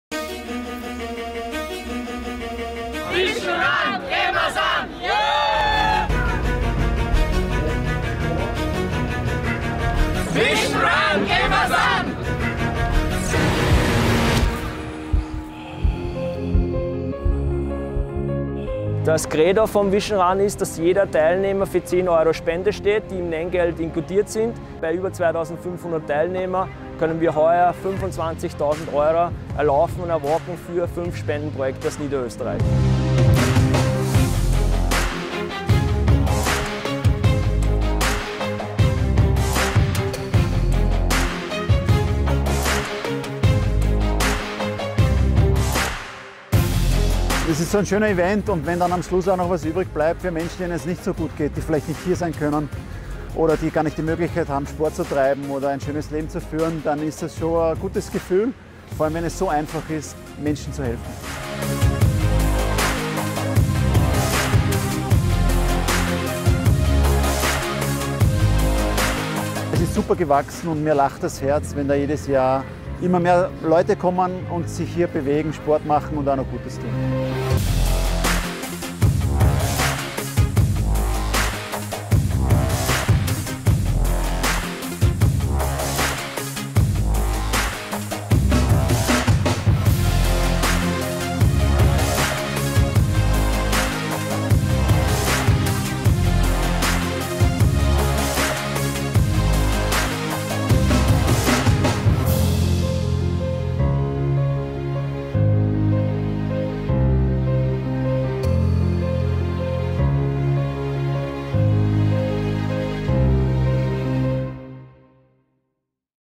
VISION RUN 2018 - 5 Jahre MEHR als ein Firmenlauf
Nicht nur, dass es mit 2.544 StarterInnen einen neuen TeilnehmerInnenrekord gibt, Nein! 848 Teams aus 125 Unternehmen und Organisationen, bestehend aus Mitarbeitern, Firmenchefs, Freunden und Kollegen habt durch ihre Teilnahme unglaubliche € 25.440,- an Spenden für die fünf Partnerprojekte des VISION RUN aufgestellt.

5 Jahre VISION RUN stehen für MEHR als einen Firmenlauf:

€ 80.260,- Spendensumme –7.896 StarterInnen – aus über 150 Firmen

So bewegt man Niederösterreich!

Wir möchten ganz laut DANKE sagen und uns bei jedem einzelnen Teilnehmer, Besucher, Helfer und Unterstützer recht herzlich bedanken! Ein besonderer Dank gilt unseren Partnern sowie unseren Sponsoren, ohne die der VISION RUN nicht möglich wäre!